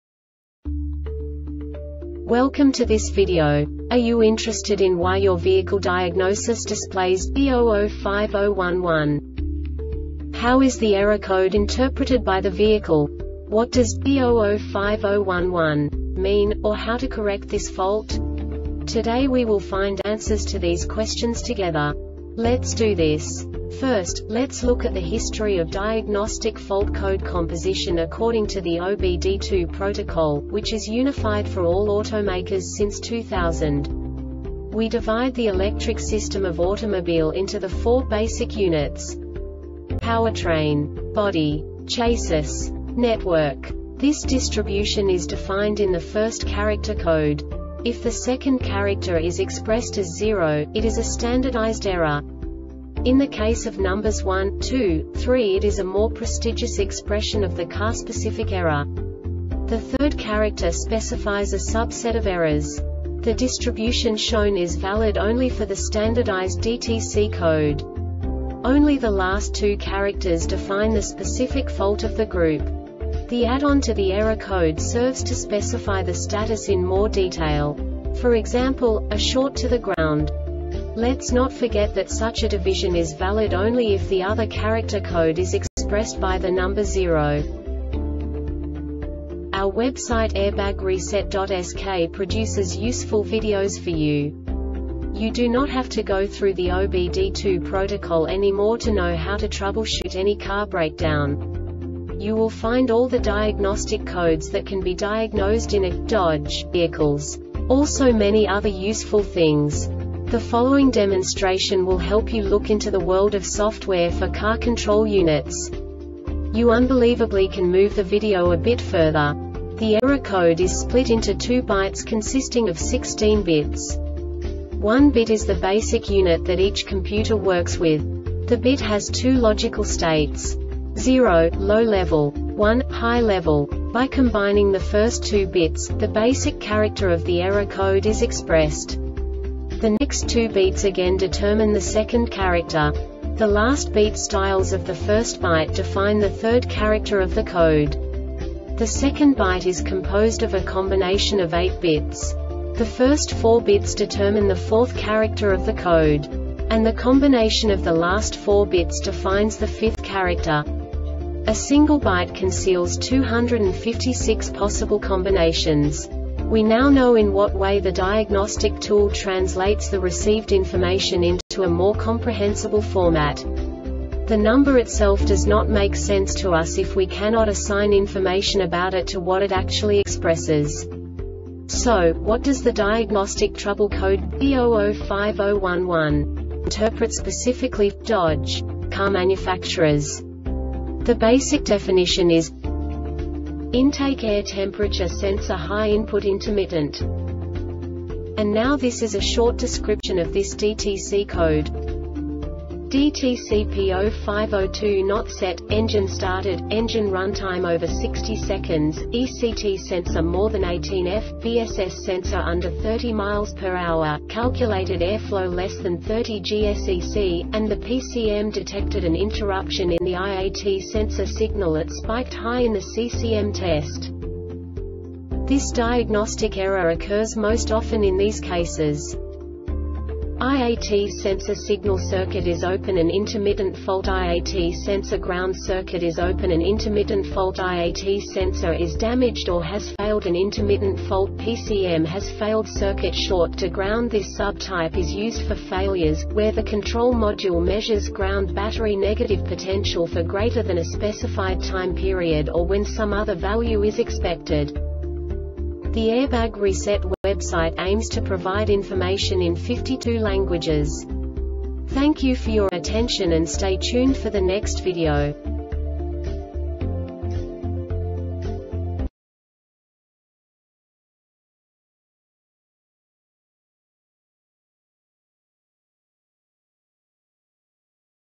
DTC Dodge B0050-11 Short Explanation
Contents:
0:21 Basic DTC analysis according to OBD2 protocol standard.
1:48 Insight into programming
2:58 A diagnostically understandable description of the Diagnostic Trouble Code
3:05 Basic error definition

Make:
Dodge
Code:
B0050 11
Definition:
Intake Air Temperature Sensor High Input (Intermittent)
Description:
DTC P0502 not set,engine started,engine runtime over 60 seconds,ECT sensor more than 18ºF,VSS sensor under 30 mph,calculated airflow less than 30 g or sec,and the PCM detected an interruption in the IAT sensor signal (it spiked high) in the CCM test.
Cause:
IAT sensor signal circuit is open (an intermittent fault)IAT sensor ground circuit is open (an intermittent fault)IAT sensor is damaged or has failed (an intermittent fault)PCM has failed
Failure Type: